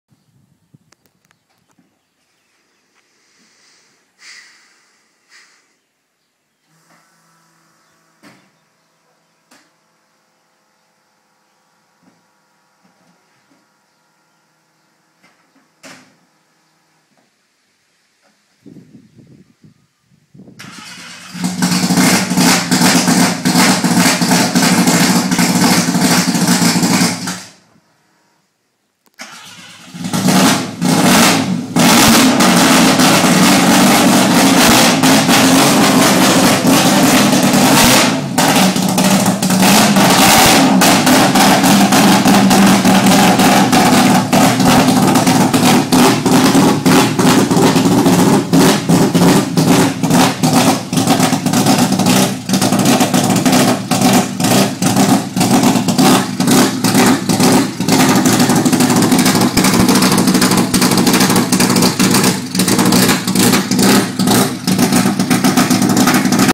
The Dazda Emerges
Martin's 12A PP powered Datto 1600 starts and moves for the first time since the engine conversion.  It's offensively loud.